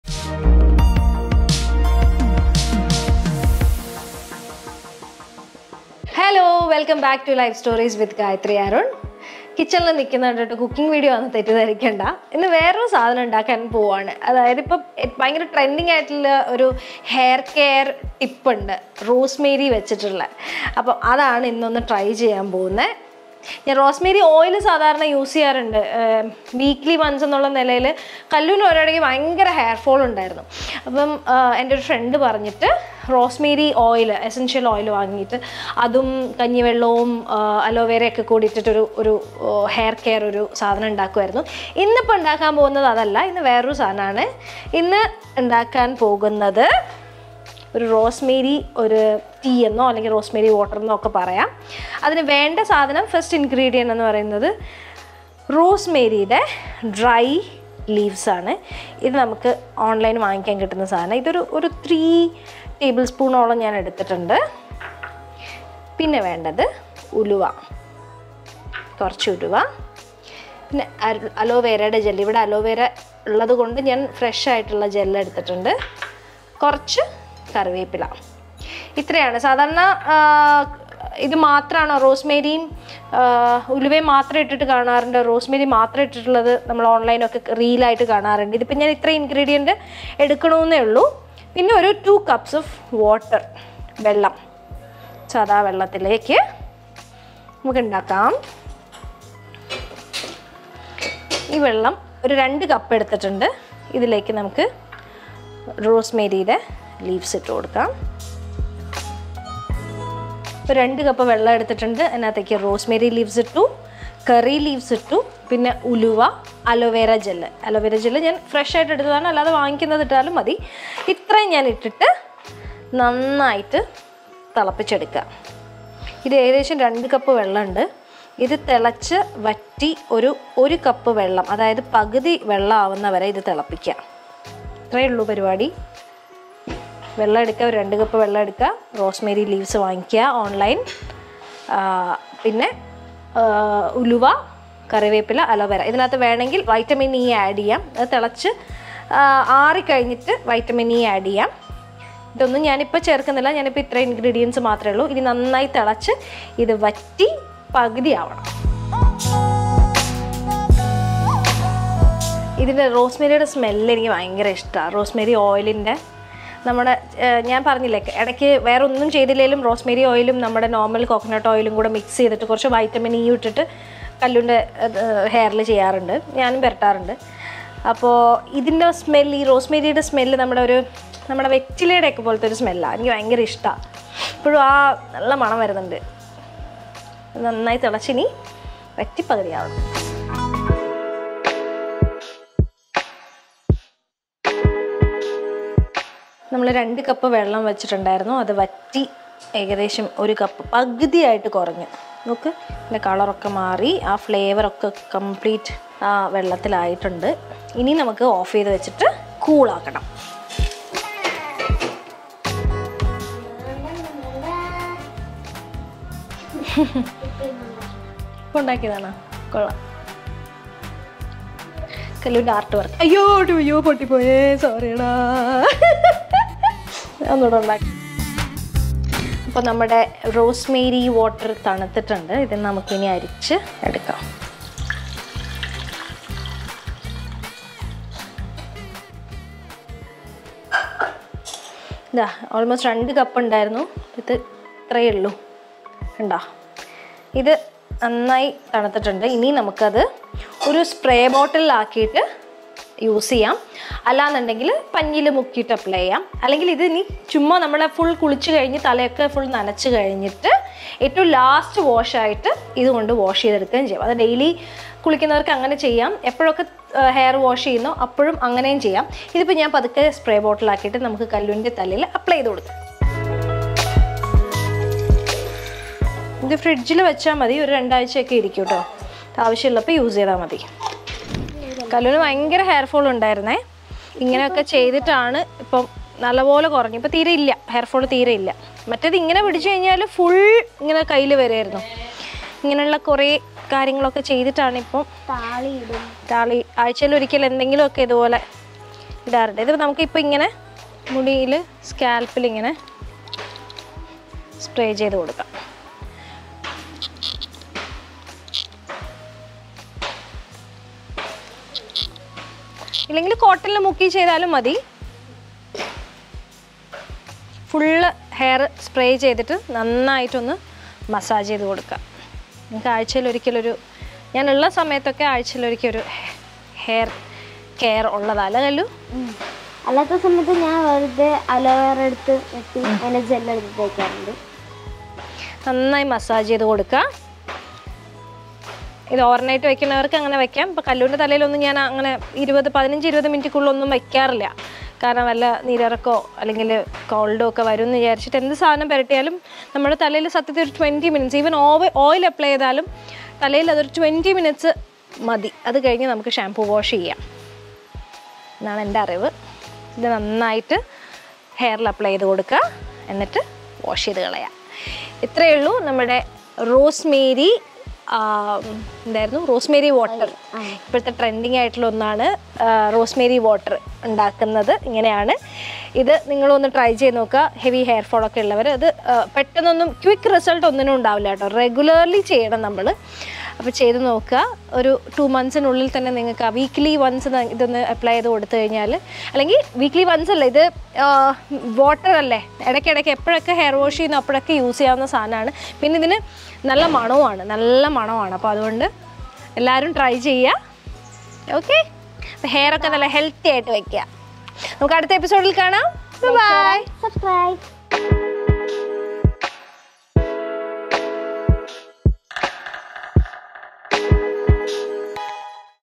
VIRAL HAIR CARE TIP | Life Stories with Gayathri Arun
Discover the Magic of Rosemary Hair Care! 🌿✨ Join me on a journey through the latest trending hair care craze - Rosemary! Learn how this fragrant herb can transform your locks, from promoting growth to adding shine. Watch as I explore DIY recipe to achieve luscious, healthy hair. Don't miss out on the secret to radiant tresses in my new YouTube vlog